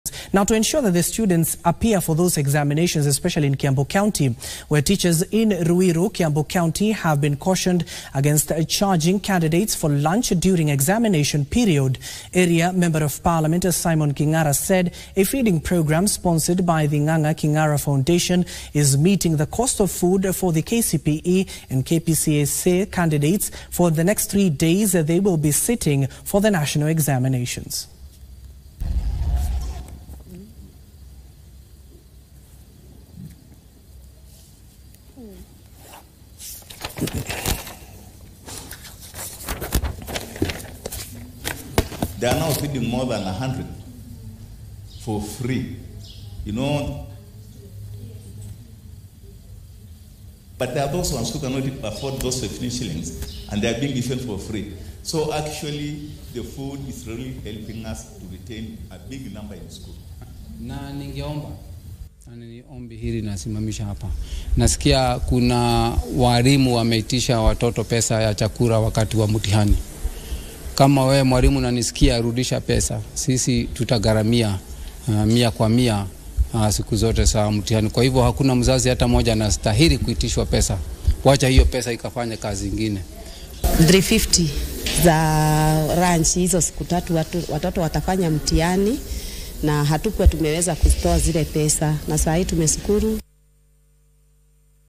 Teachers in Ruiru, Kiambu have been cautioned against charging candidates for lunch during exams
Teachers in Ruiru, Kiambu County have been cautioned against charging candidates for lunch during the examination period.